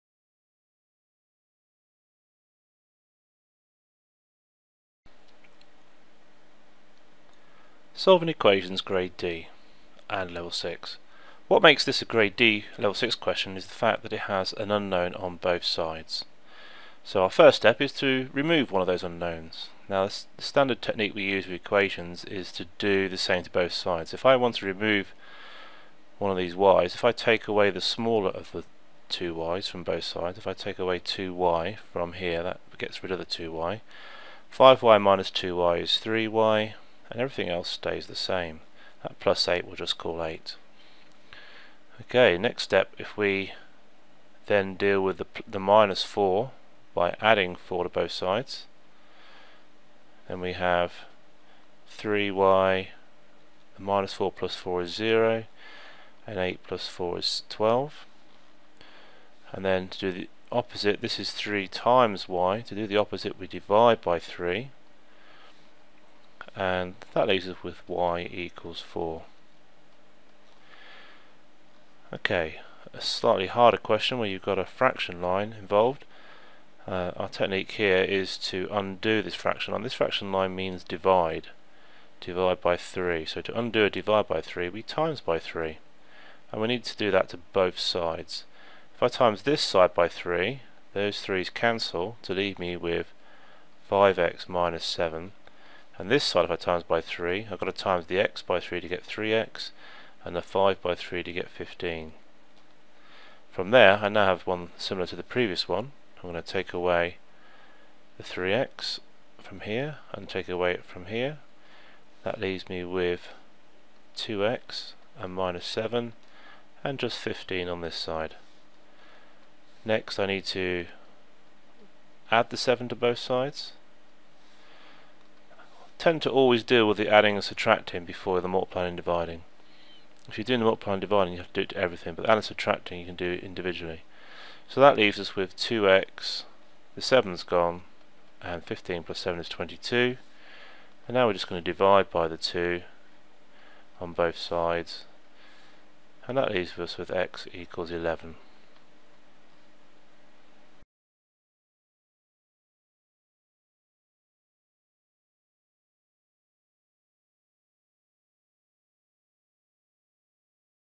Solving linear equations, GCSE Maths revision Exam paper practice
AQA Linked Paired Pilot Methods 2 Higher Practice Paper Question 1
Solving simple equations, although part (b) is a bit trickier